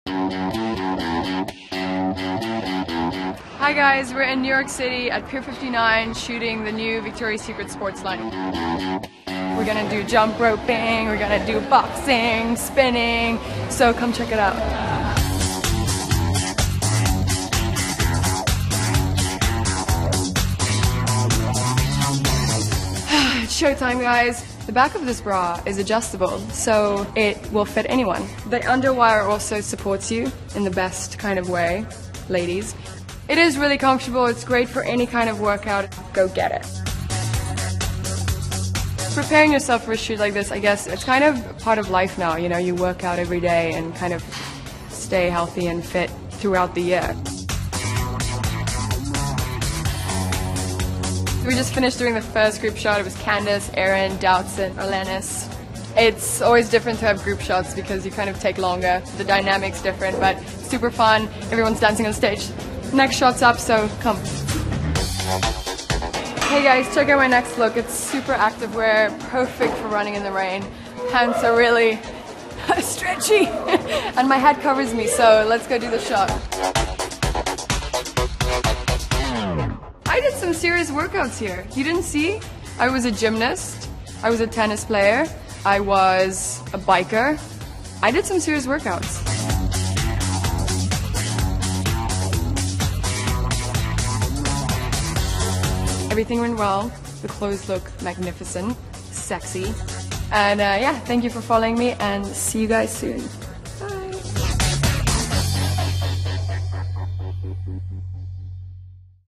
VSX Sport: Behind the Scenes of the Holiday 2012 Shoot
Oct 1, 2012 - Join Victoria's Secret Angel Behati Prinsloo on the set for an inside look at the Holiday 2012 shoot for the VSX Sport Collection. Also featuring Arlenis Sosa, Candice Swanepoel, Doutzen Kroes and Erin Heatherton.